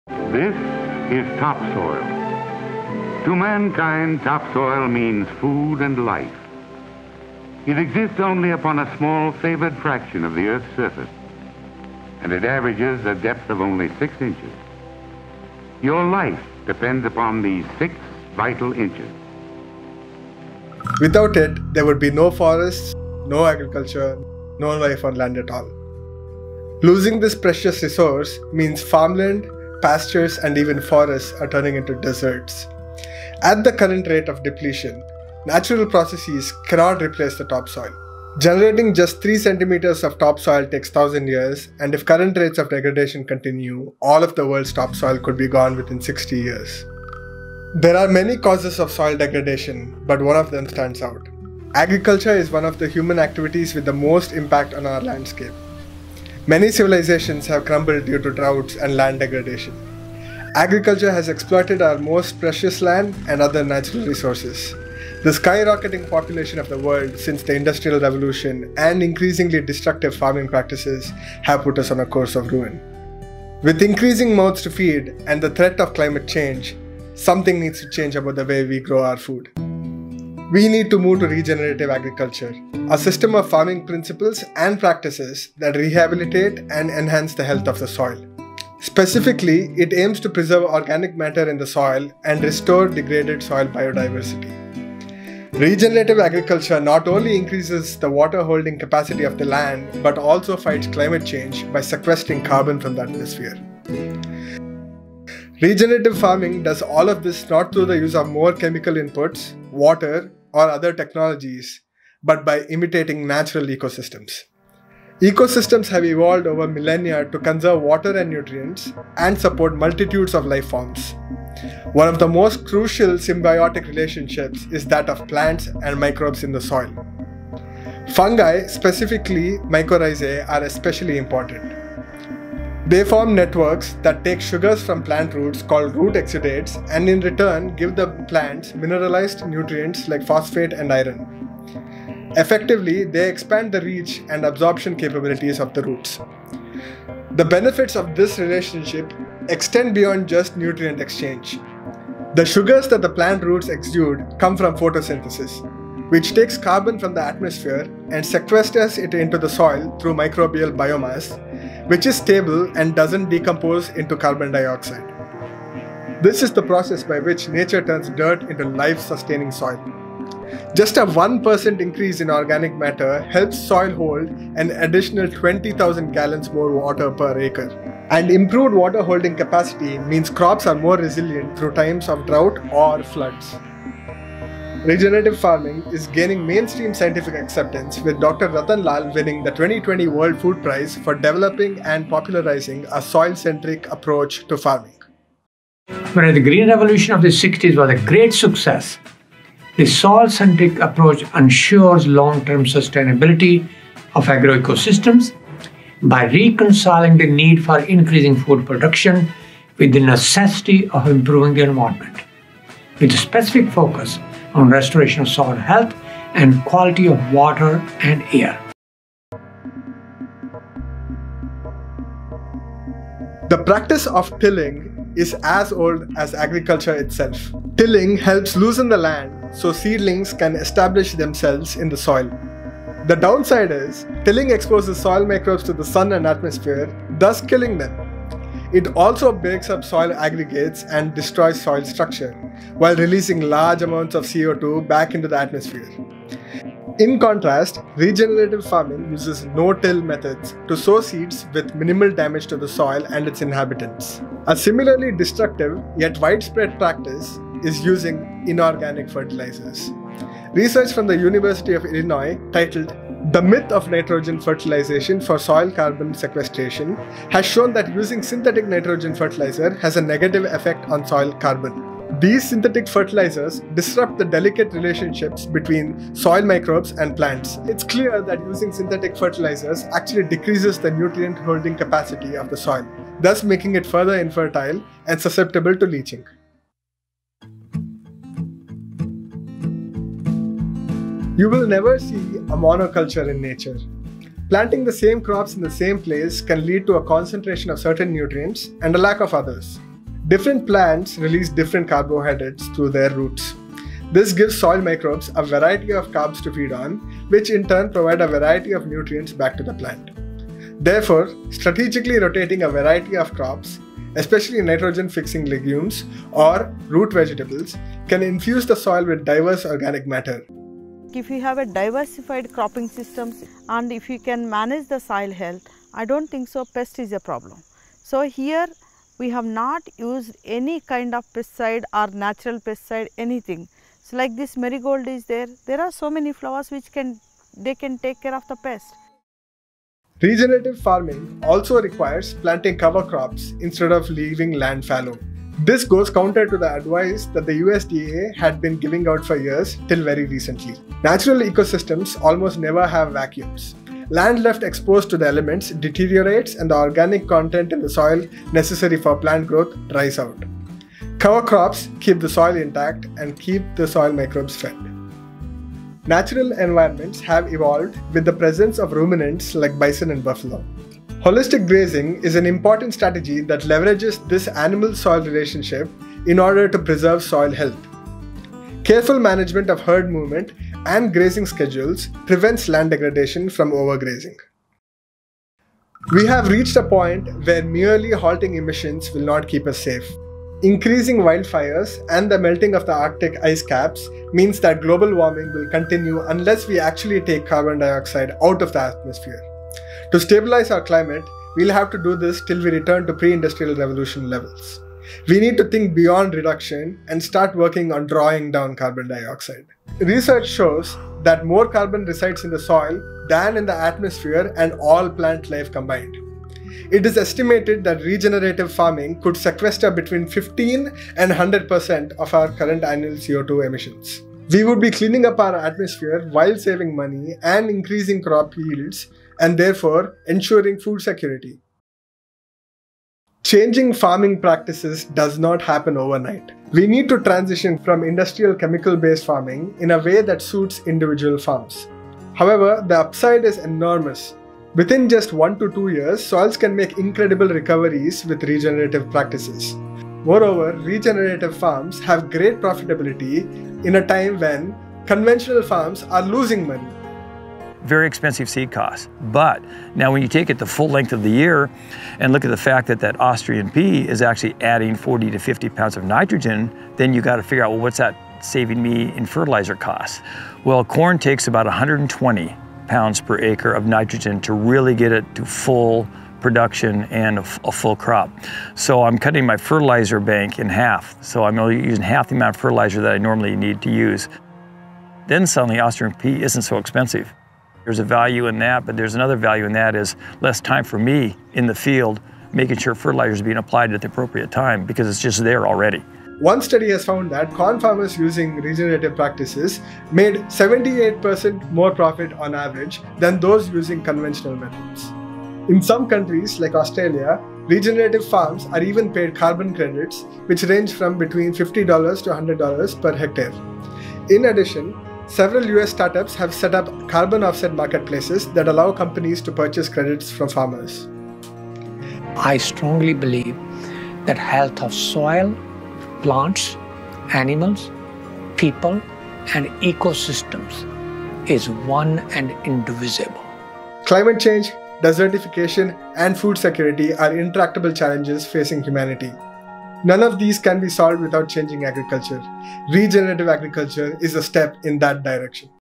Regenerative Farming: A Climate Change Solution
Regenerative Farming aims to conserve soil health by building up soil organic matter through increasing microbial biodiversity. Regenerative practices not only increase long term health of the soil and increase farm profits but also reverse climate change by sequestering carbon from the atmosphere into the soil.